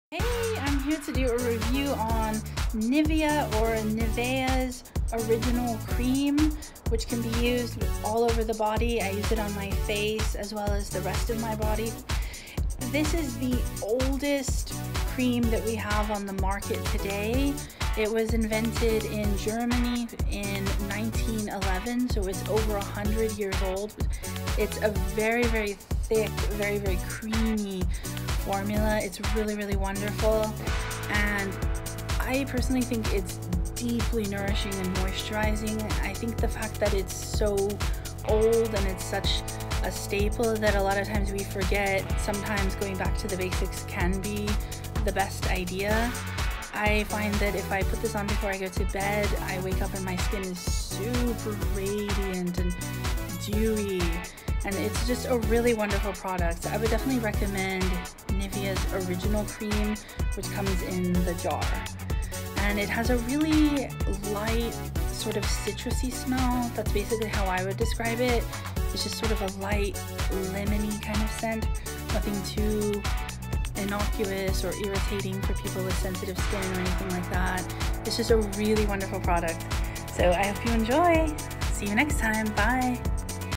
Nivea original cream review
This is a review of the original cream by Nivea invented in Germany in 1911. The name is taken from the Latin word Nivis which means snow.